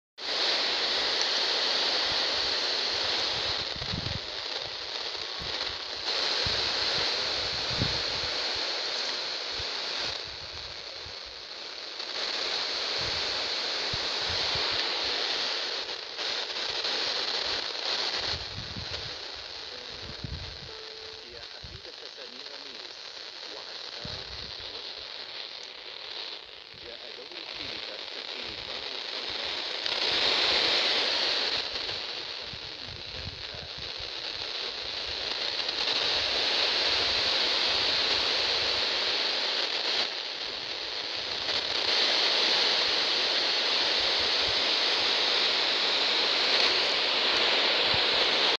[Es] 92,3 MHz - Zed Radio - Arbil (1767 km)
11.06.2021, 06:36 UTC
QTH: Semiluki, Voronezh region, Russia
Equipment: Radiwow R-108, antenna built-in-whip+external telescopic